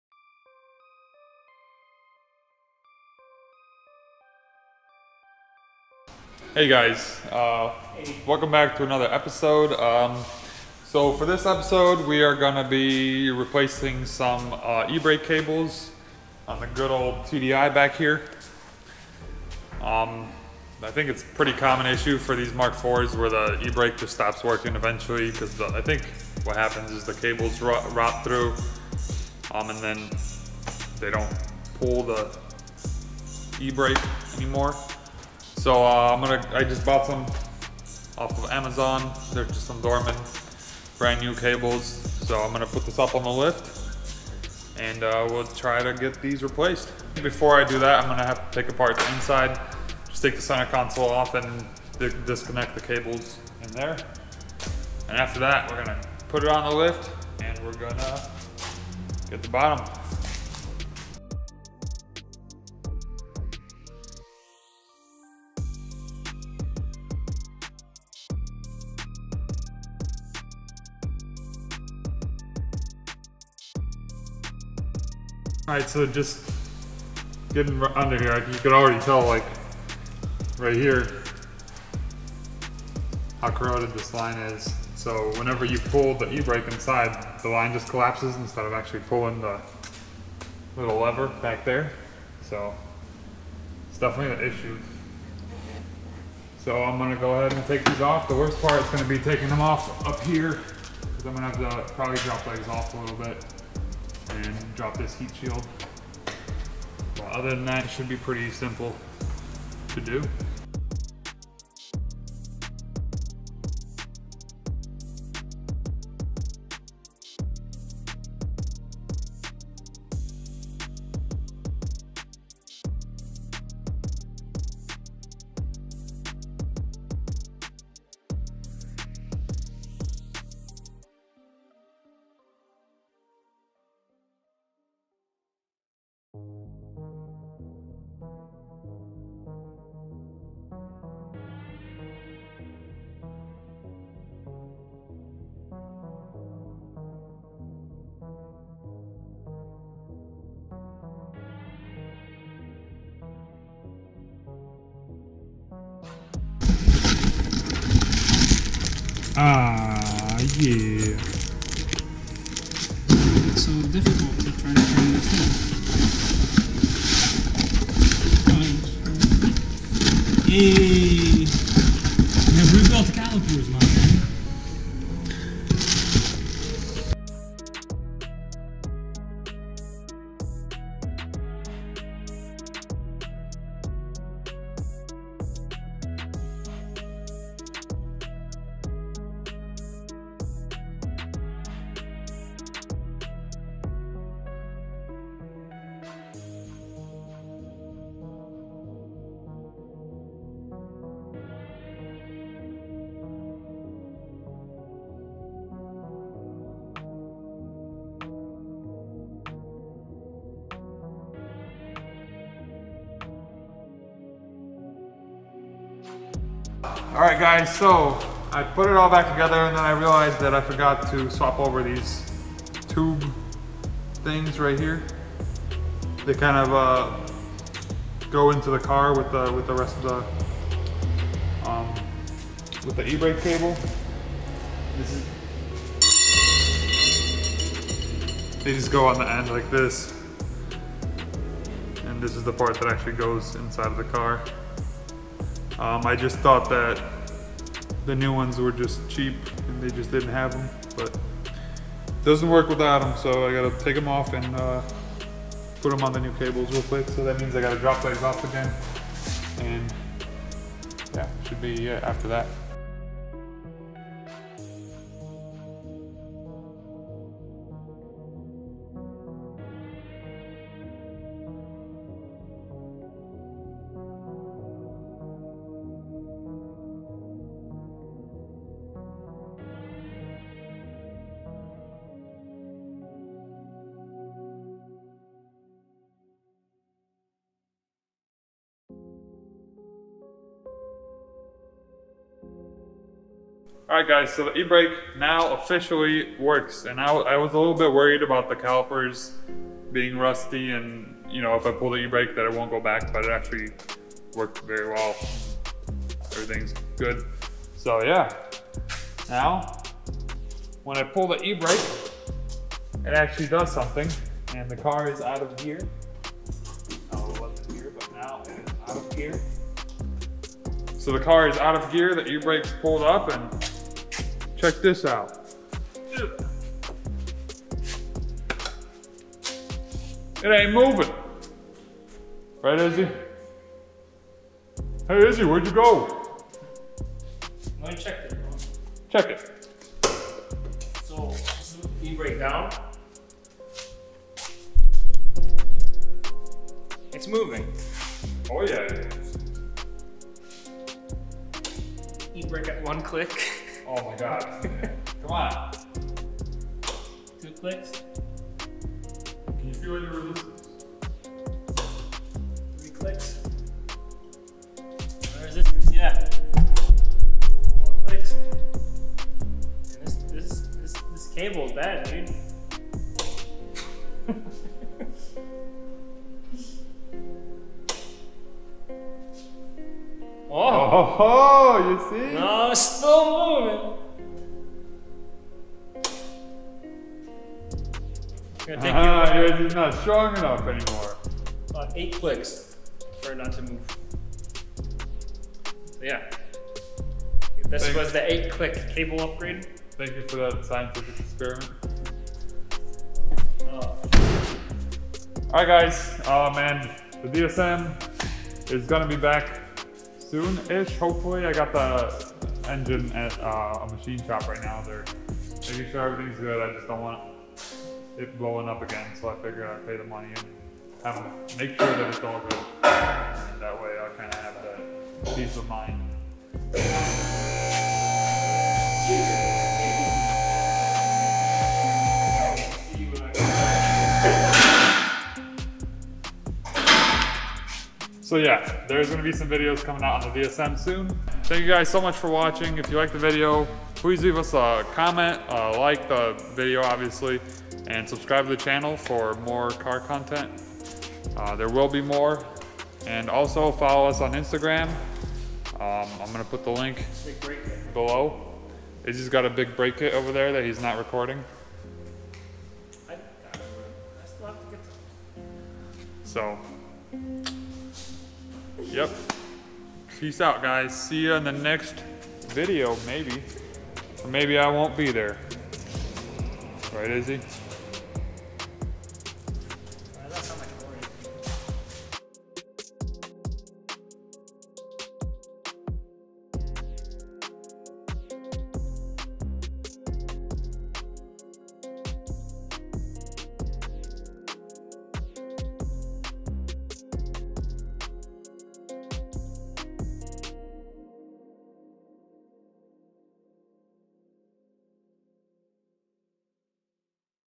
Mk4 VW E-Brake Fix
The Volkswagen MK4 platform is getting old. Many of these cars have common issues that just occur naturally after these cars get old. We personally have experience with at least 10 of these cars where the emergency brake does not do anything at all. This video shows us fixing the most common cause (in our opinion) that the emergency brake does not work on our 2005 VW Jetta. This cause is rusted out emergency brake cables. These cables have a couple of metal clips that hold them to the car, and with time, as they move around, the plastic protective coating wears off and leave the metal inside exposed to water and salt. This results in the metal rotting away which takes away the ability of the cable to engage the emergency brake.